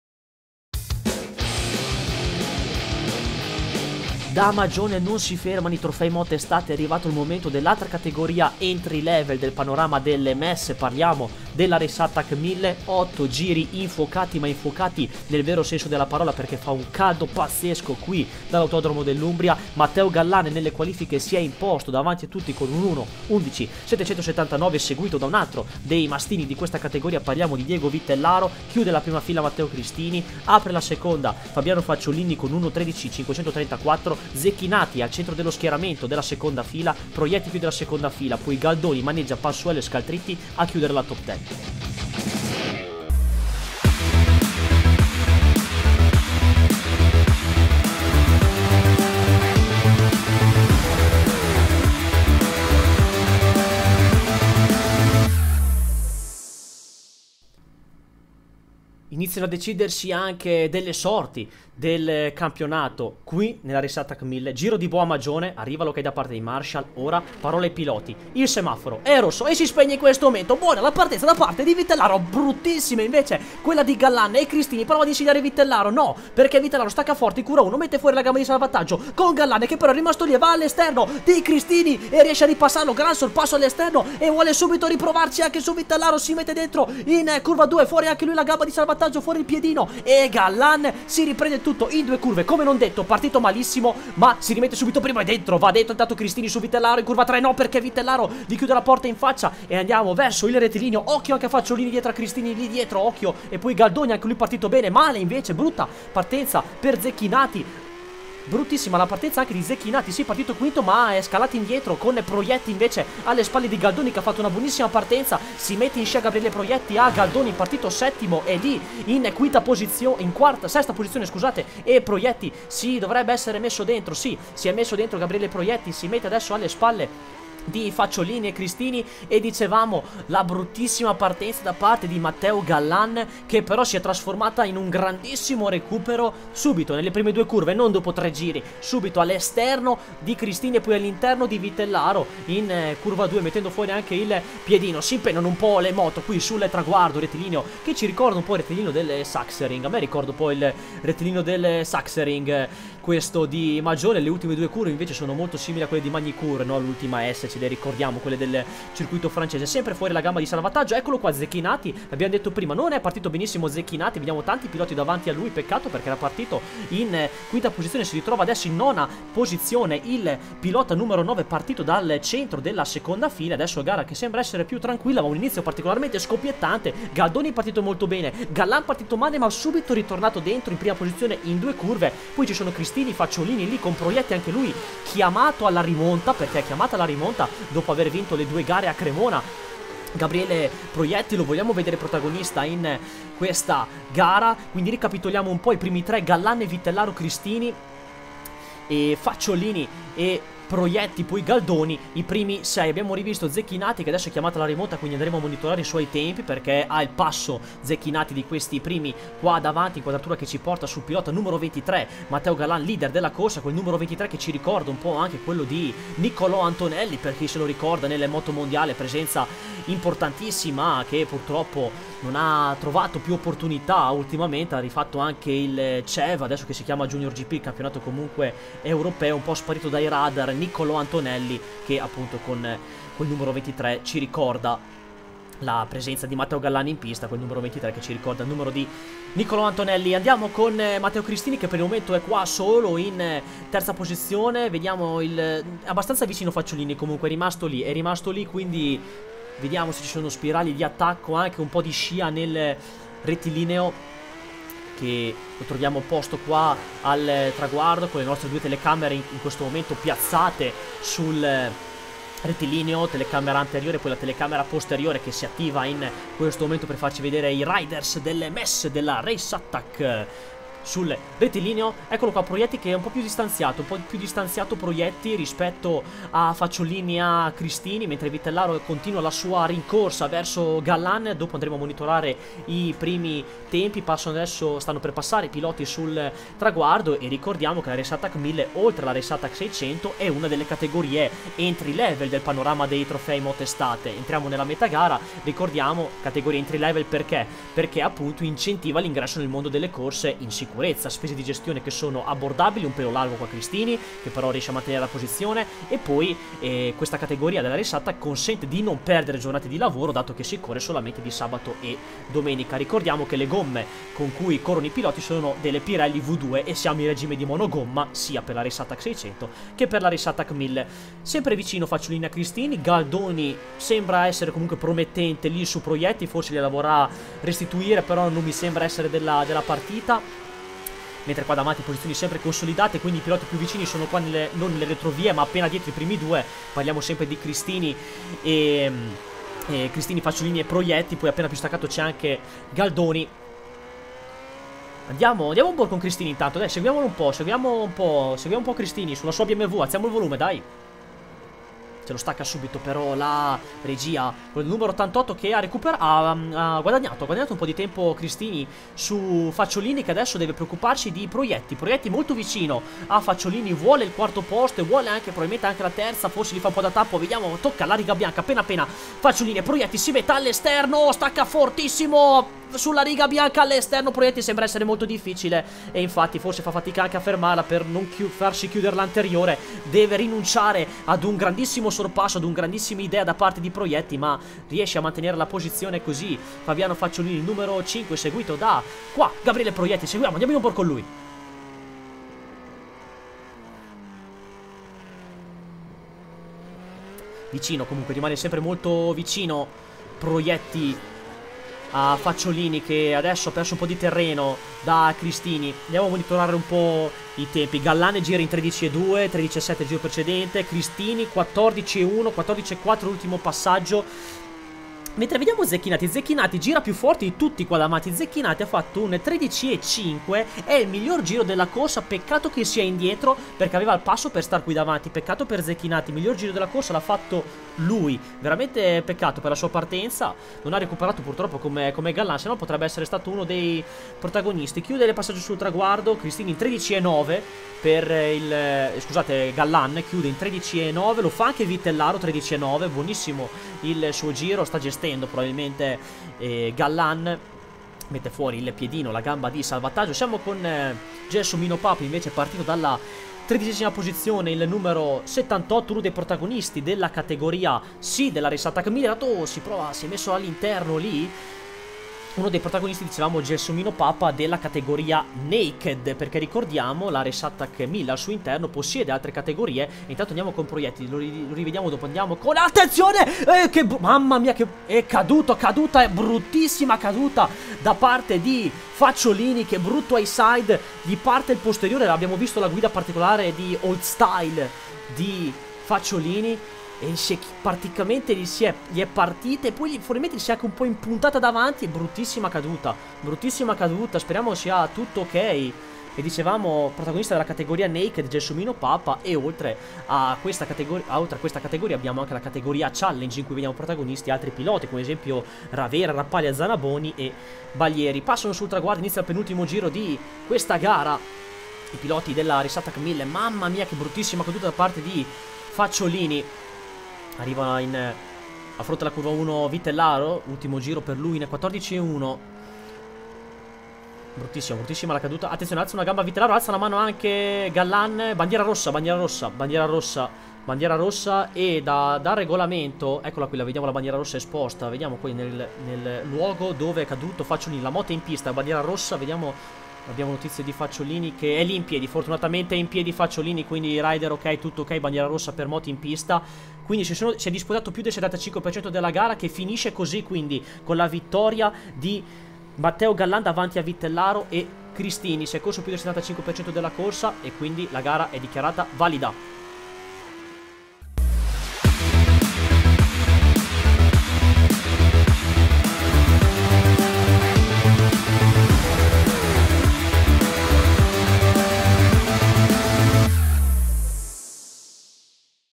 MES, R03: RACE ATTACK 1000 CON UN FINALE INCREDIBILE A MAGIONE💥😎
Succede di tutto nella Race Attack 1000, la categoria più partecipata dei Trofei WRS MES 2024! Le gare sono sempre imprevedibili e ricchi di colpe di scena e a Magione non è stato da meno!